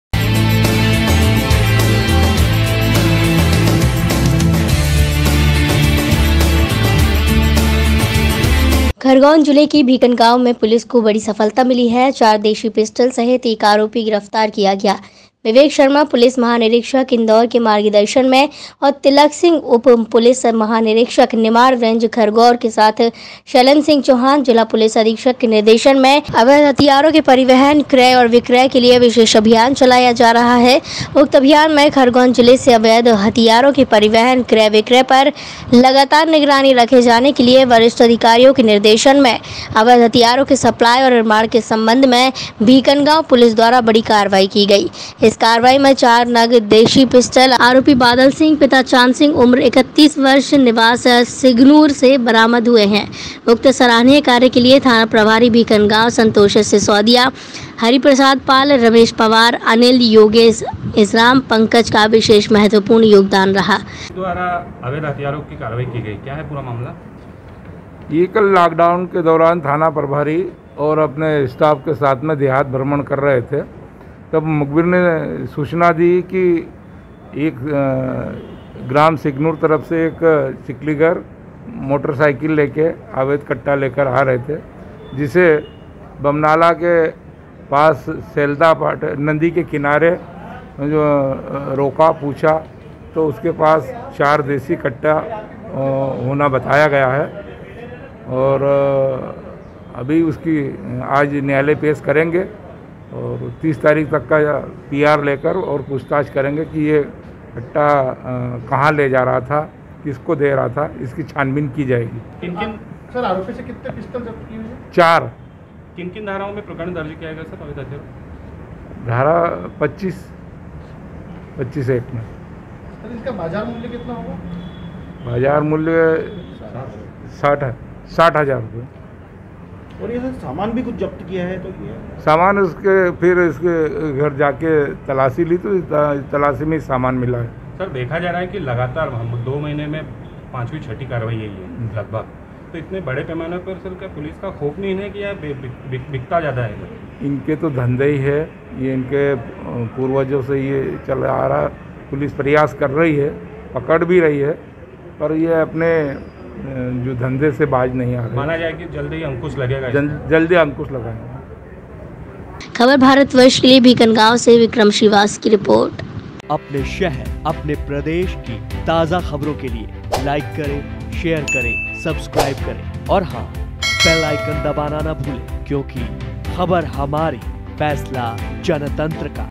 भीकनगांव : 04 देशी पिस्टल सहित 01 आरोपी गिरफ्तार , घर से अवैध कट्टे बनाये जाने वाली सामग्री वरामद
भीकनगांव : 04 देशी पिस्टल सहित 01 आरोपी गिरफ्तार आरोपी के घर से अवैध पिस्टल कट्टे बनाये जाने वाली सामग्री भी वरामद